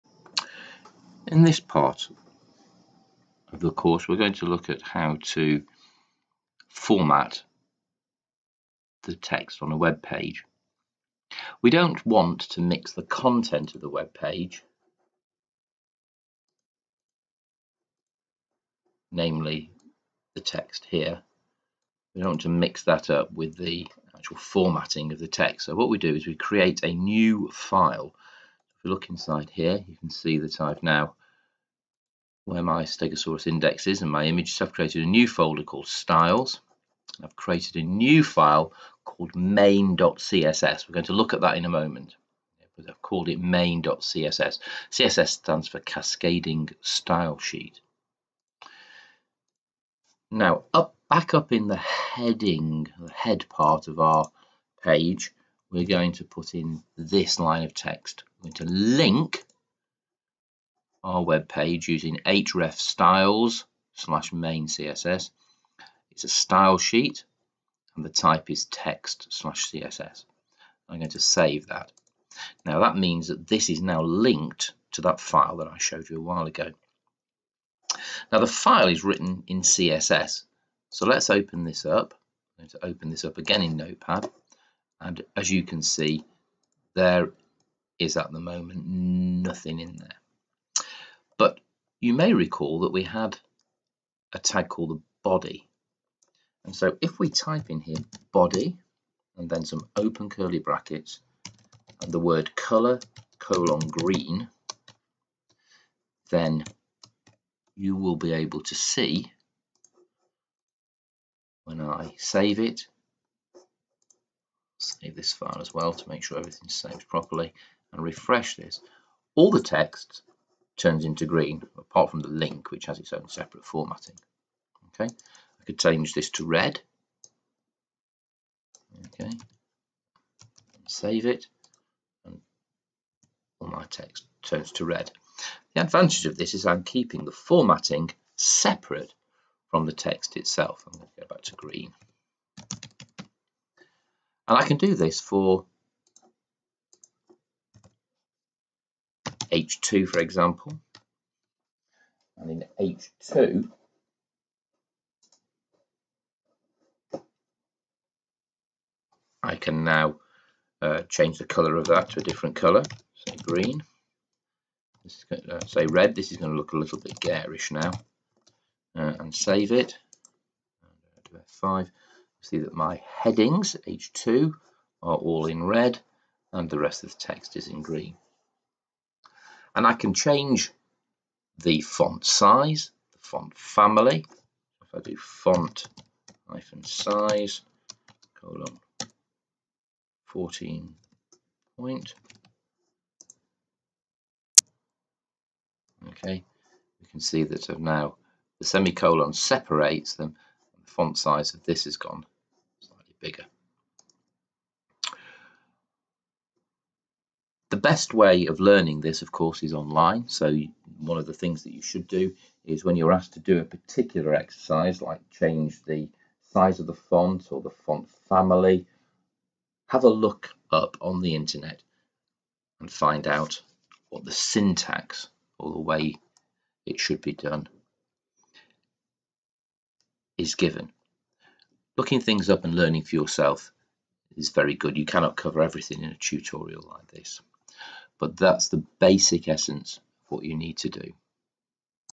css styles
How to format an HTML web page using css styles sheets.